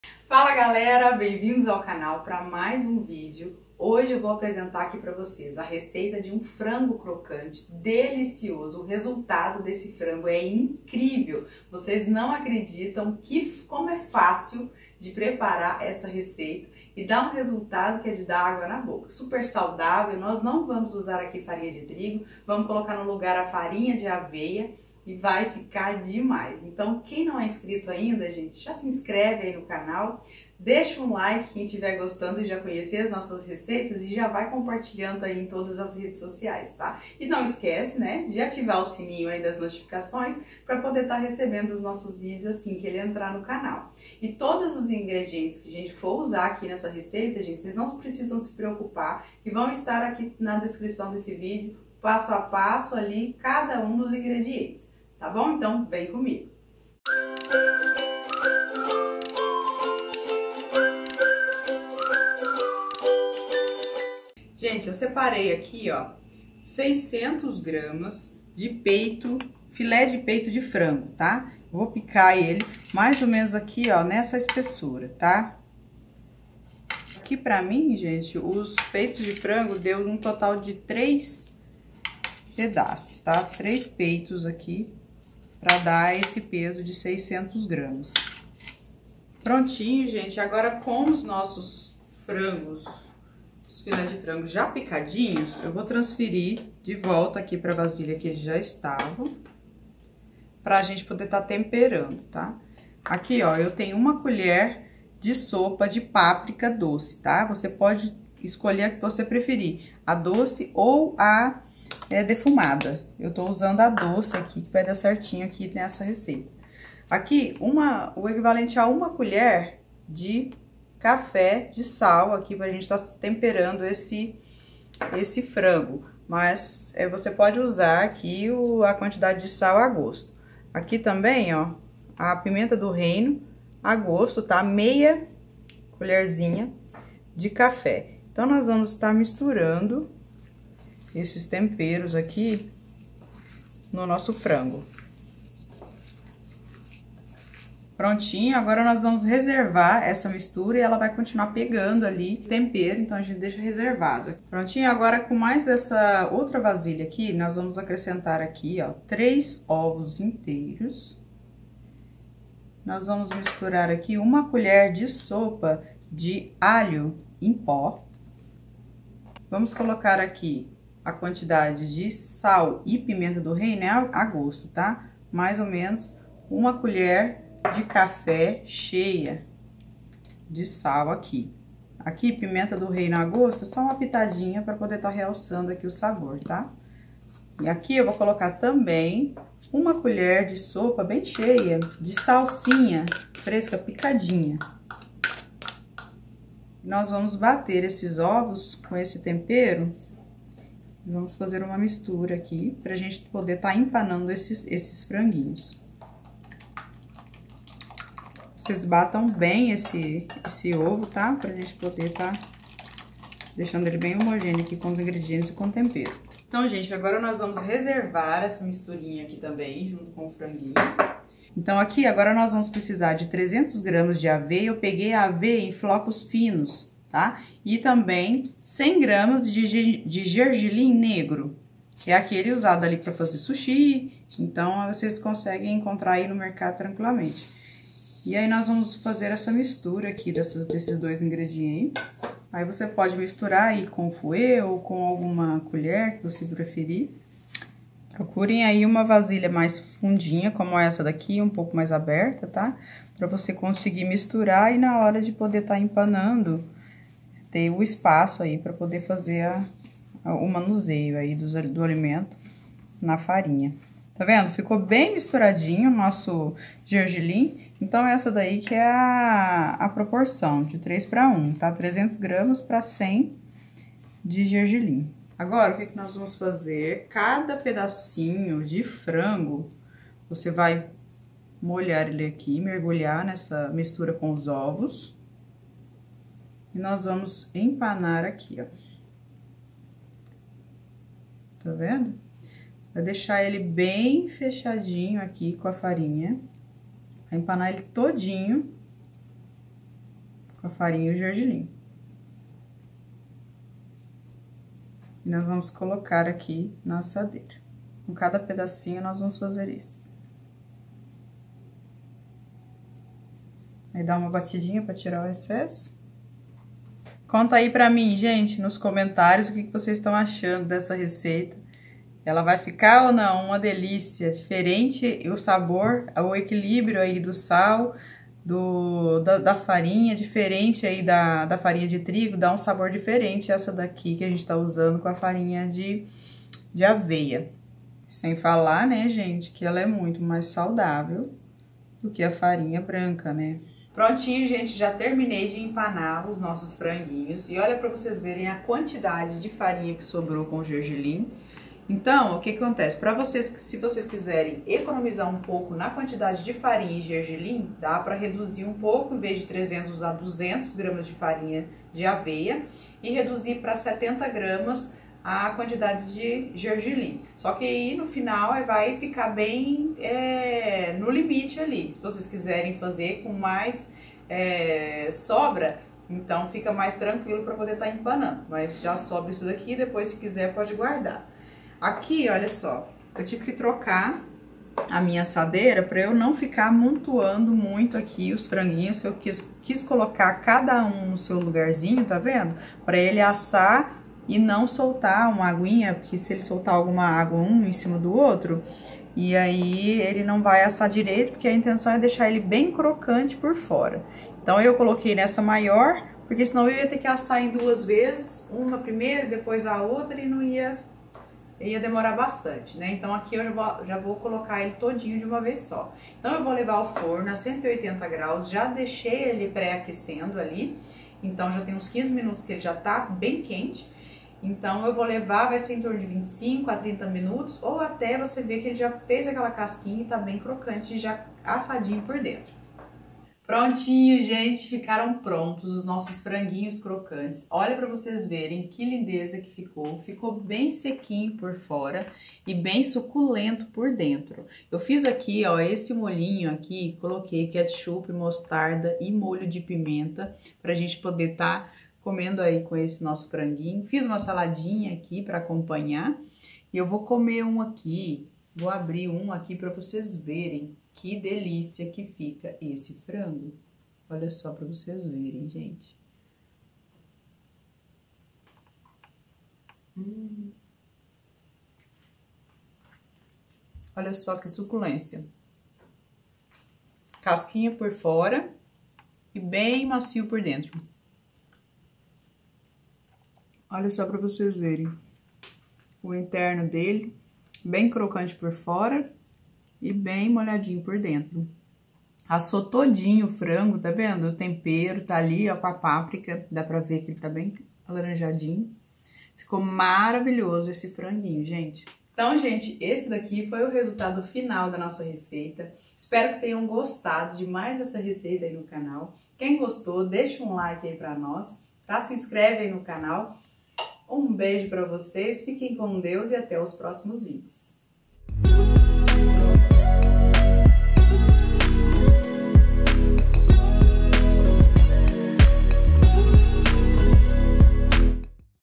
❤ FRANGO CROCANTE como você nunca viu, Ficou DEMAIS!!! Cozinha da Danni
❤ Frango Crocante como você nunca viu, Ficou DEMAIS!!!
Bom dia meus amores🥰, estou chegando com uma receita deliciosa pra vocês de como fazer um frango crocante delicioso, crocante por fora e macio e suculento por dentro, essa receita de frango crocante está um arraso, sabe aquele frango super fit e saudável empanado com farinha de aveia que não chega nem perto do óleo, pois é, esse não é um frango empanado comum, porque essa receita não é frita e sim assada, não é demais. Essa receita de frango crocante é muito fácil e rápida de fazer, além de muito diferentes das receitas de frango comuns que nós costumamos fazer do dia a dia, vamos experimentar? Vocês vão adorar este frango crocante empanado e assado bem temperado na medida certa para o seu aperitivo!
Acompanhem o passo a passo e abaixo estão listados todos os ingredientes utilizados nesta receita.
Beijos 😘 e espero que gostem de mais esta receita, fiquem com Deus e até os próximos vídeossss. 💃💃💃

INGREDIENTES:
✔  600g de frango
✔  1 colher de sopa de páprica
✔  3 ovos
✔  1 colher de sopa de alho em pó
✔  1 colher de sopa de salsinha
✔  300g de farinha de aveia
✔  100g de gergelim negro